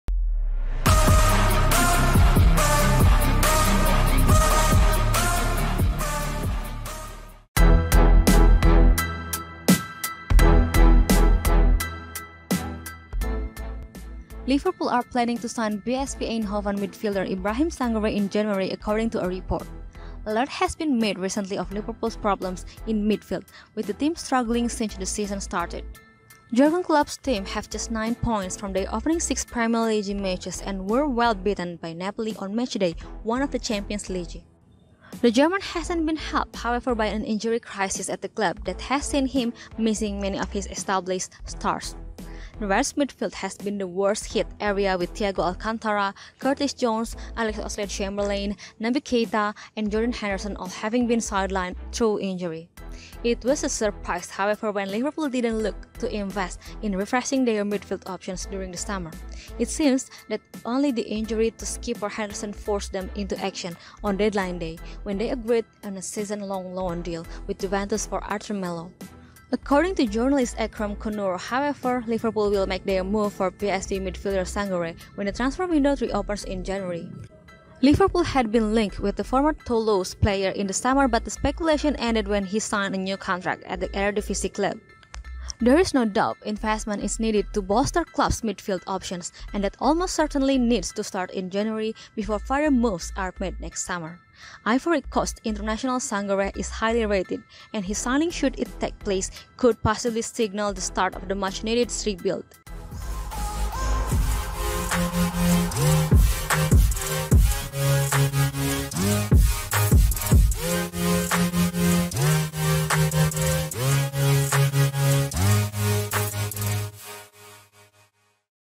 REPORT❗ Liverpool Plan To Sign Ibrahim Sangare In January Transfer
Liverpool are planning to sign PSV Eindhoven midfielder Ibrahim Sangare in January according to a report.

A lot has been made recently of Liverpool's problems in midfield with the team struggling since the season started.

Jurgen Klopp's team have just nine points from their opening six Premier League matches and were well beaten by Napoli on matchday one of the Champions League.

The German hasn't been helped however by an injury crisis at the club that has seen him missing many of his established stars.